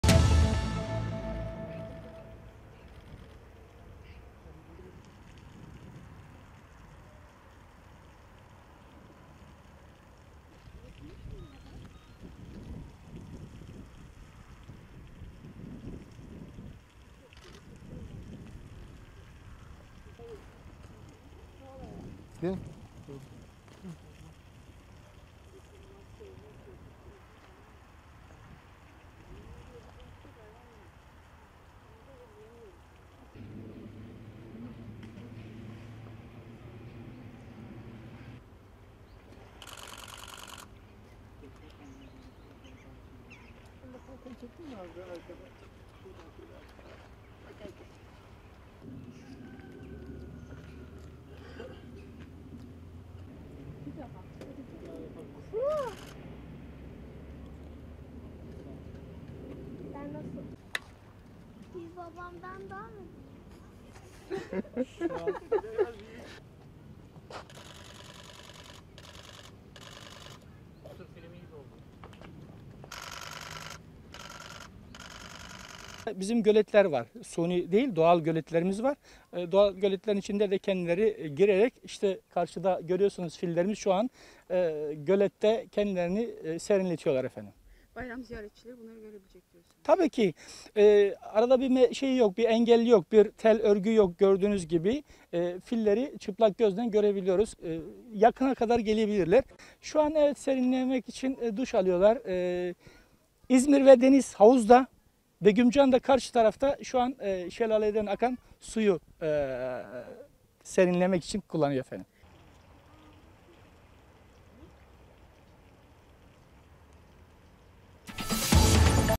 Fil ailesi de bayram temizliğinde
İzmir Doğal Yaşam Parkı'ndaki 130 türden bin 500'ün üzerinde hayvan bayram misafirlerini ağırlamaya hazırlanıyor, parktaki en renkli görüntülerden birini de Asya fillerinin yaşadığı bölgede üstlerinden akan su ile duş aldığı sahneler oluşturuyor.